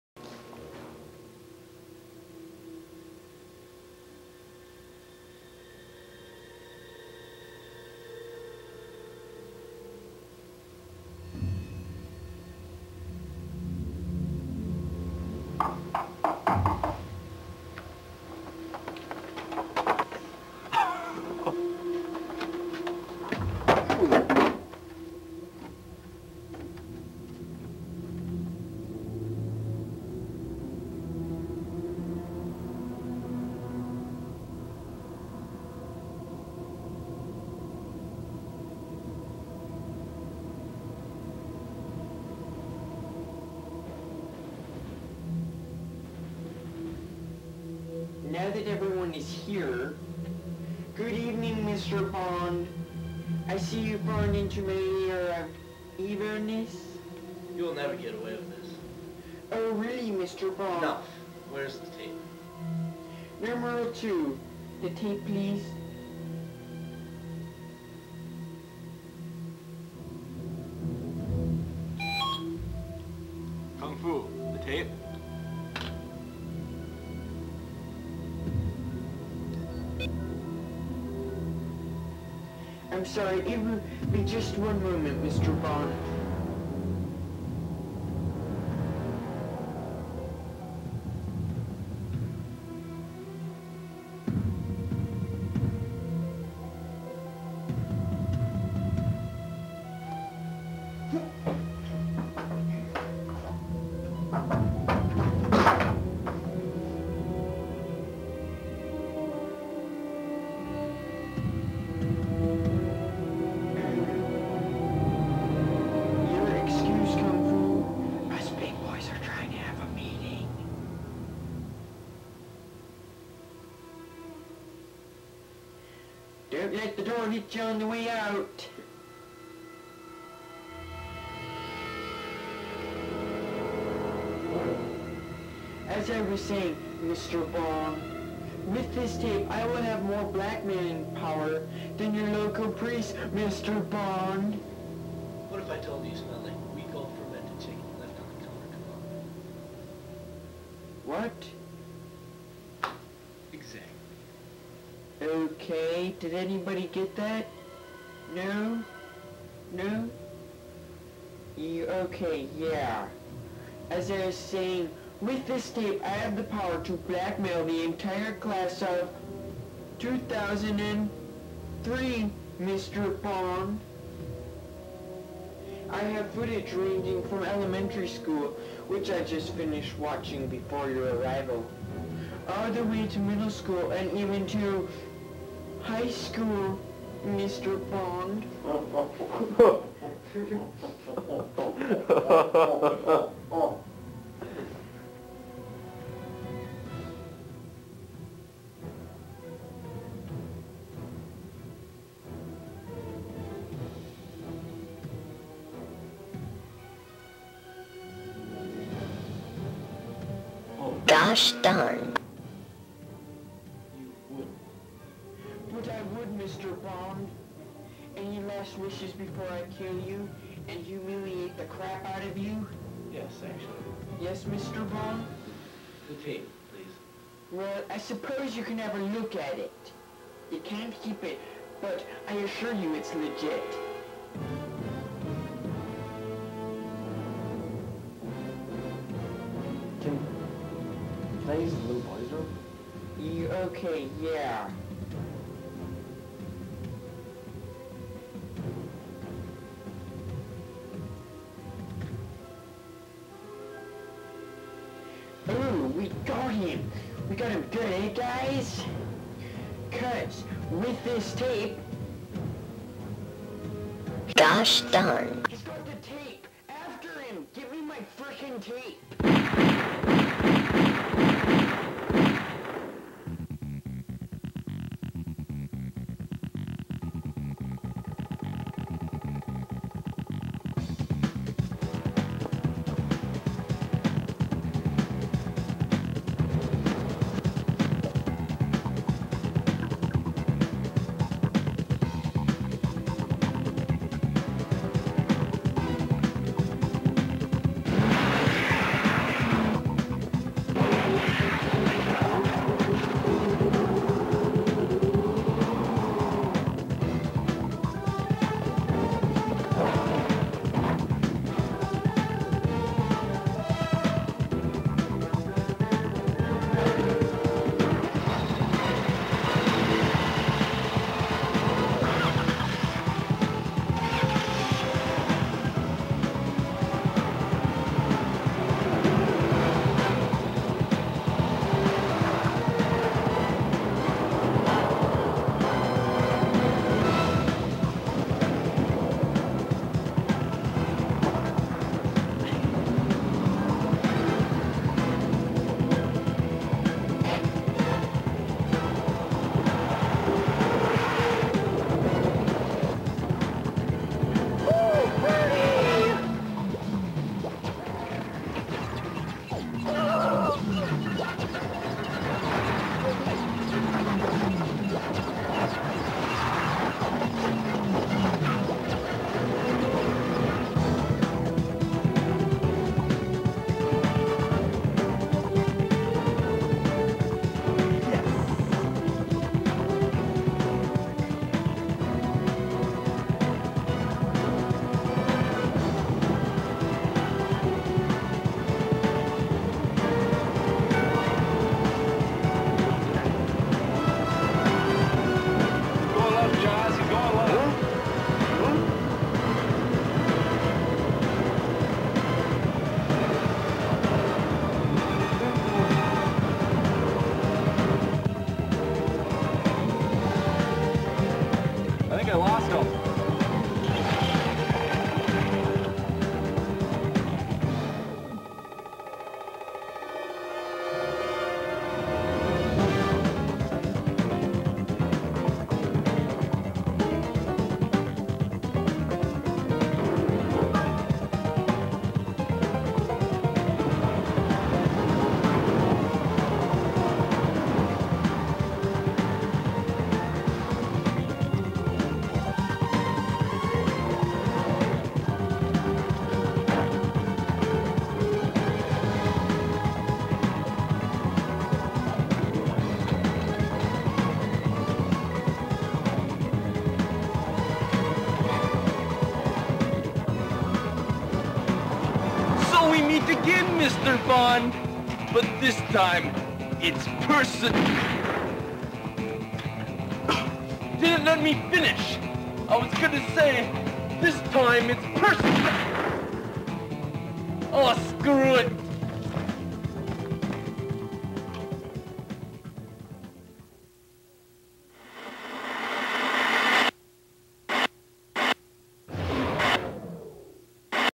OHS Senior Video 2003 - James Bond Scene Part 2
This is the second part of the James Bond scene from the Osceola High School senior video of 2003